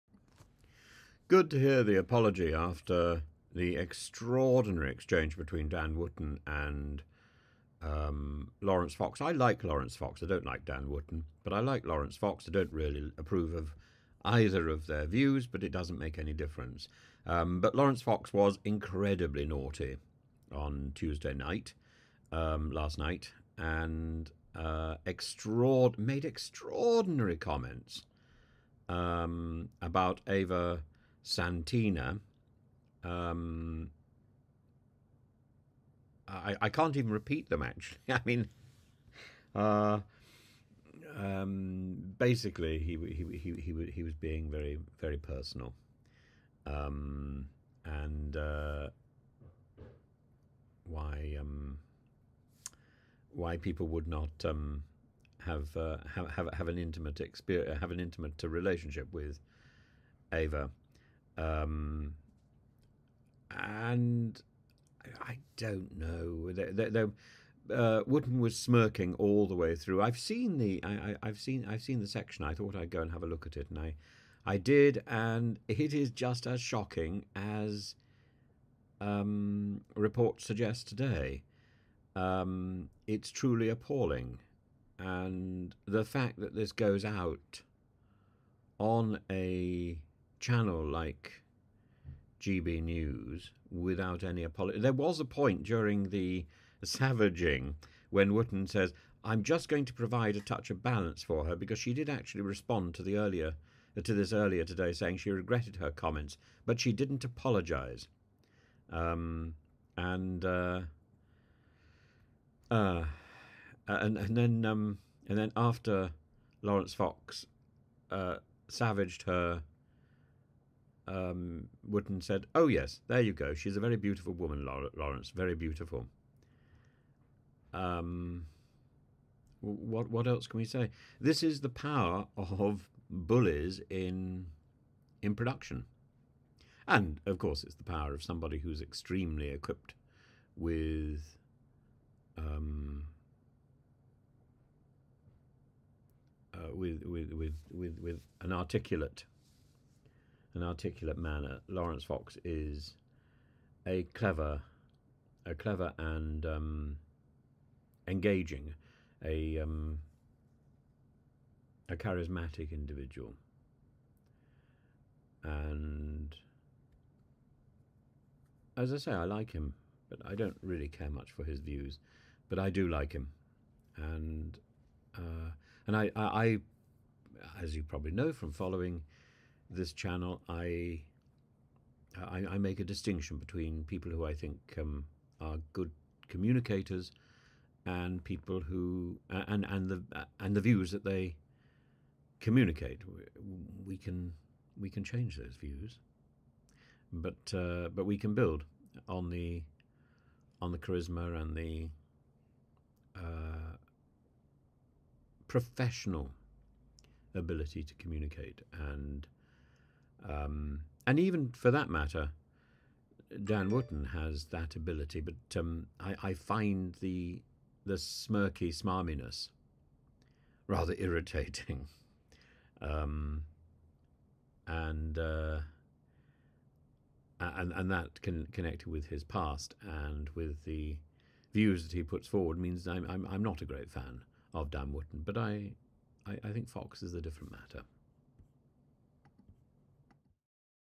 Laurence Fox delivers a scathing rant
shocking and targeted- and permitted by GBNews and a smirking Dan Wootton. Laurence should know better! I don't know whether Ava Evans should also be addressed as Ava Santina? Apologies if I got the nomenclature confused...